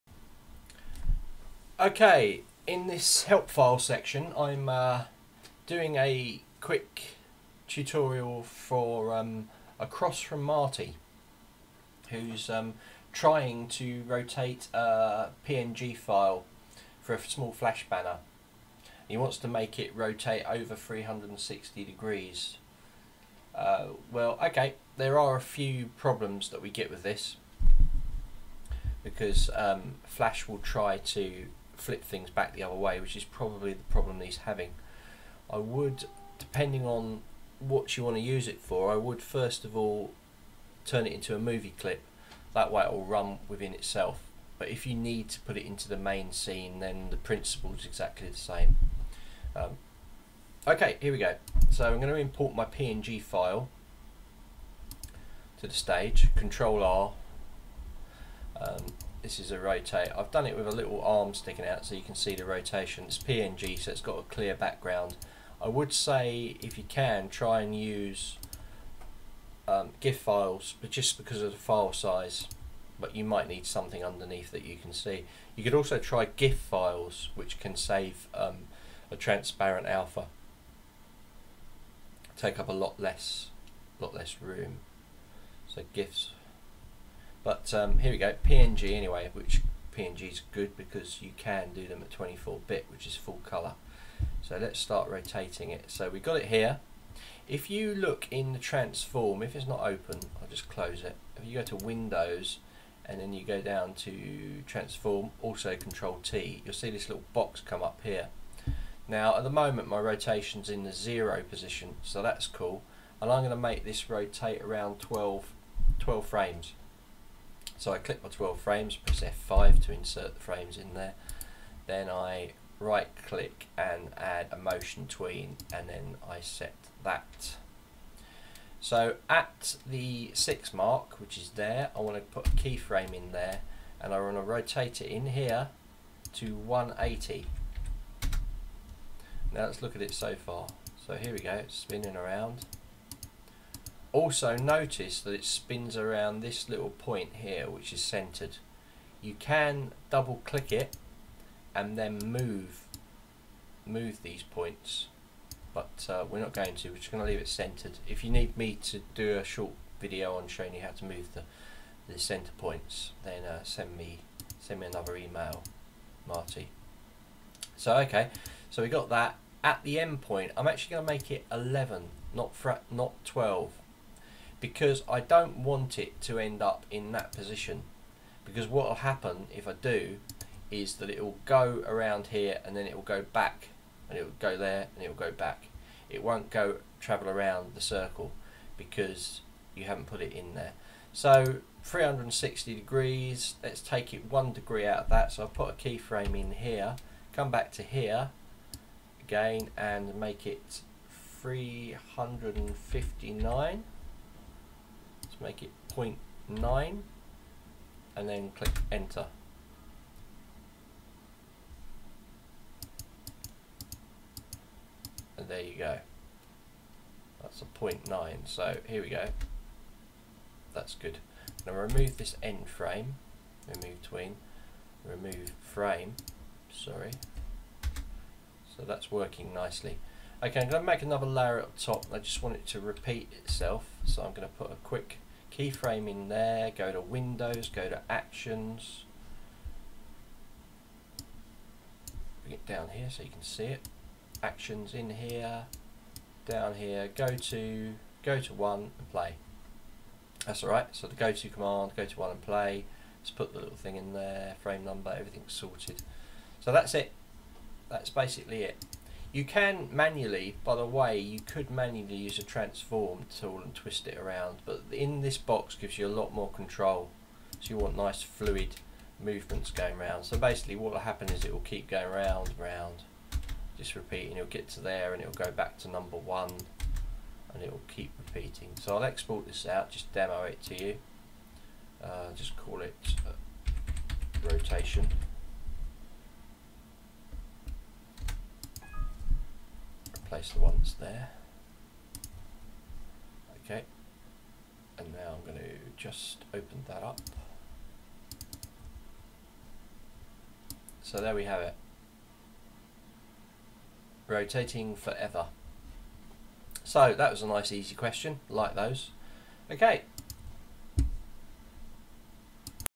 FLASH CS3 - Help1-Rotations
This is a help file for anyone having problems with Flash Rotations.